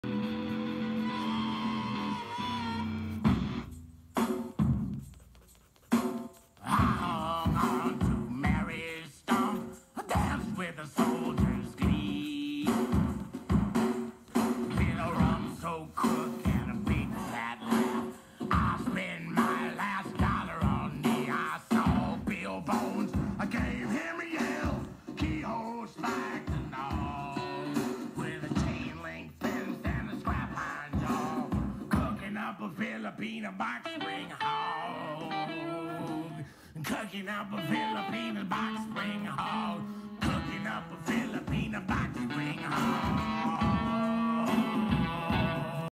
Wild Jungle pig sketch
Nothing like a bit of Tom Waits in full 'hobo-crooner mode' to get you in the mood for sketching a moody old jungle pig!